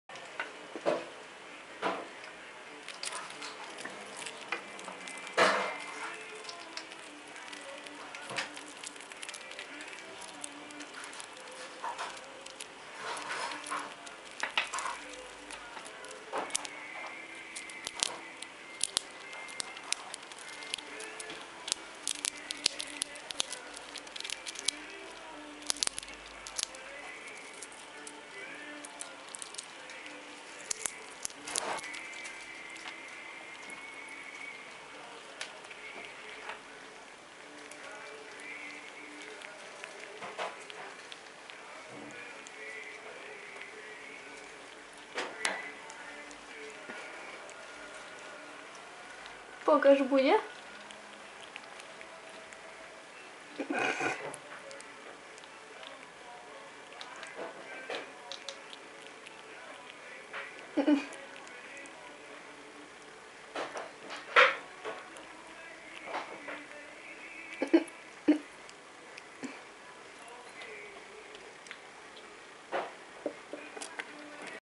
Fred niczego nie odmawia / burunduk
mój mały łasuch chomikujący wszystko co zdąży złapać lub ukraść ;]

głuptasek próbuje ułożyć pestkę żeby ta trzecia mu się jeszcze do buzi zmieściła ;)